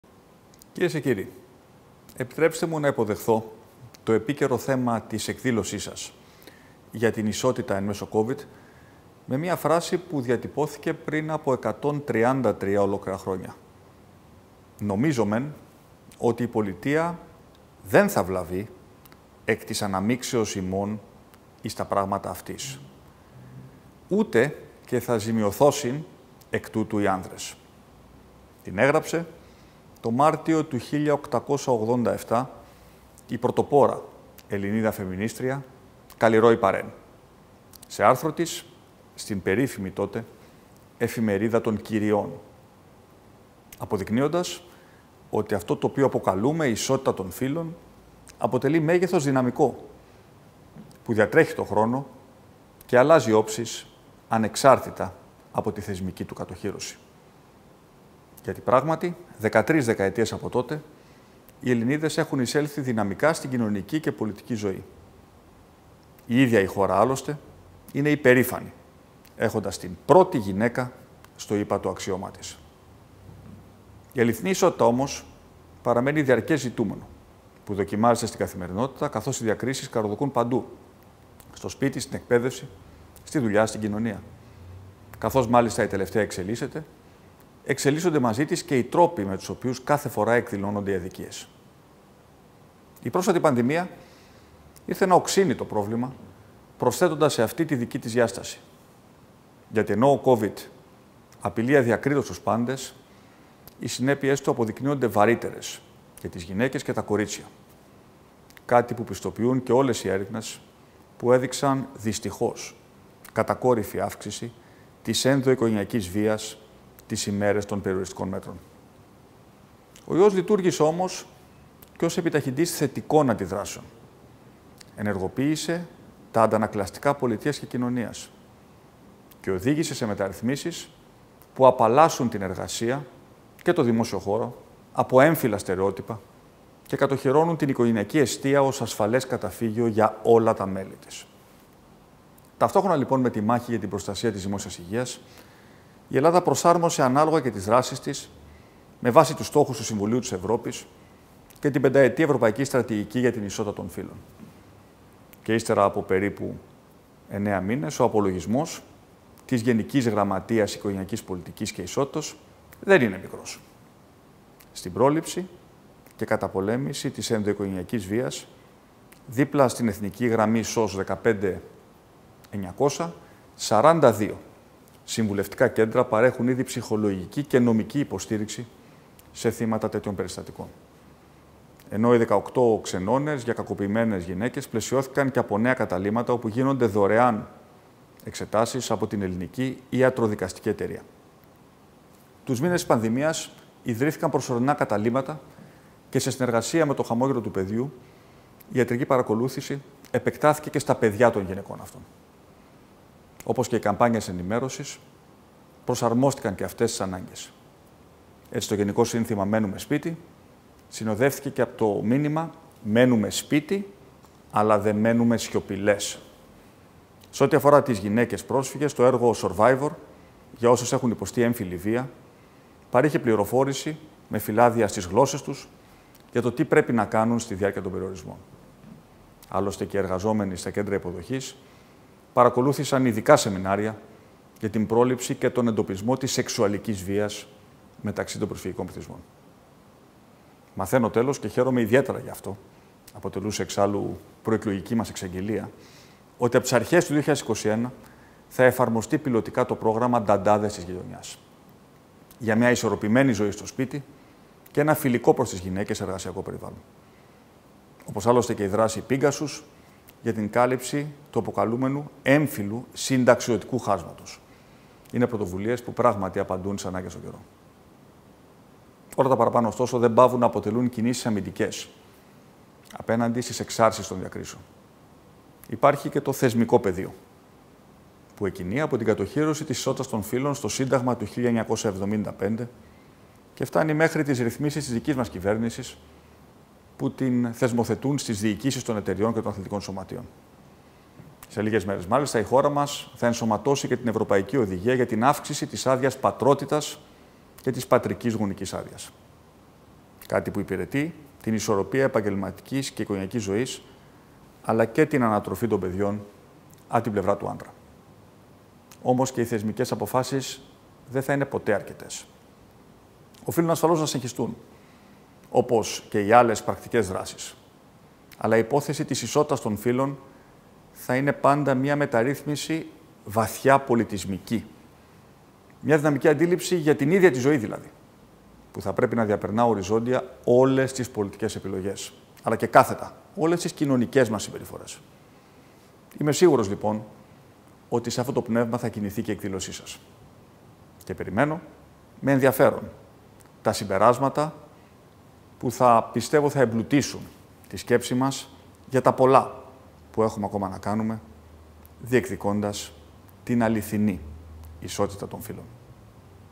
Χαιρετισμός Κυριάκου Μητσοτάκη σε εκδήλωση για την προώθηση της ισότητας των φύλων εν μέσω COVID
Χαιρετισμός του Πρωθυπουργού Κυριάκου Μητσοτάκη στην εκδήλωση «Προωθώντας την ισότητα των φύλων: ο ρόλος και το πλαίσιο λειτουργίας των θεσμικών μηχανισμών για την ισότητα εν μέσω COVID-19».